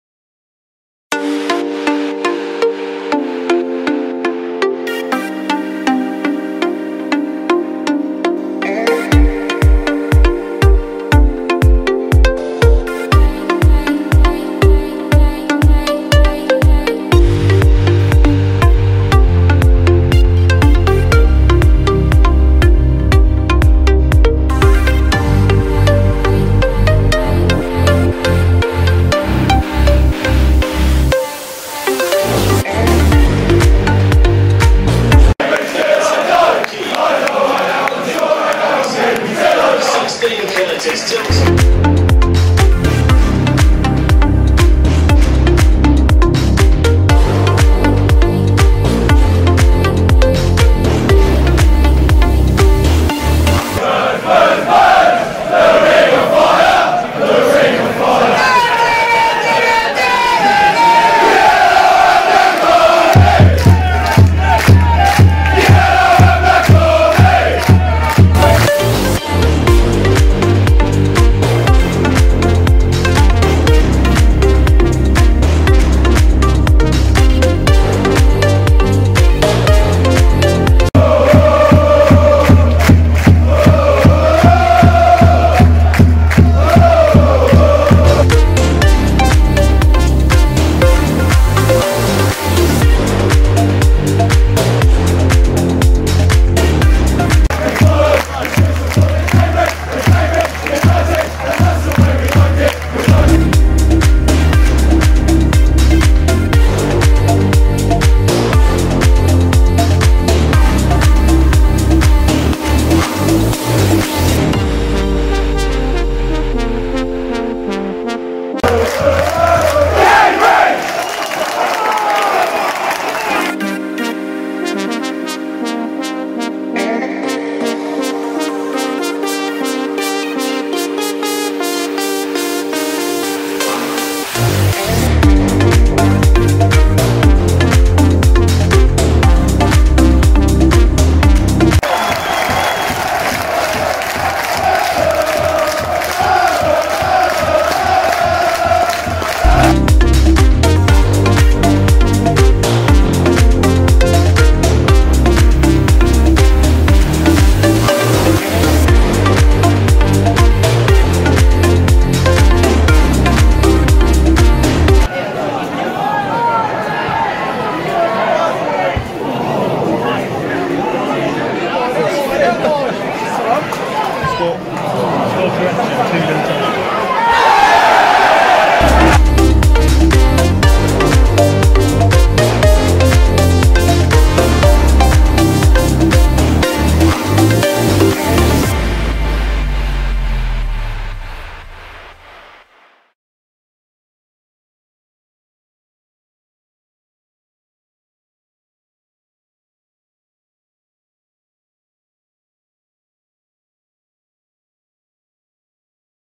Cambridge United v Wigan Athletic, 23.11.2021. A Visitor's View.
My visit to The Abbey Stadium for the 3rd tier of English Football League One game, Cambridge United v Wigan Athletic on the evening of 23rd November 2021. This is the 79th stadium I've now seen a game at out of the 92 league grounds. Hope to complete the set one day.

Pictures & Video: Huawei P30 Pro mobile phone